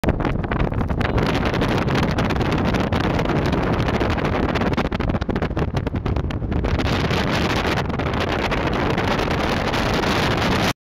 Potato camera goes flying
Potato camera (canon s95) goes flying from bavallen. About 2004 quality video.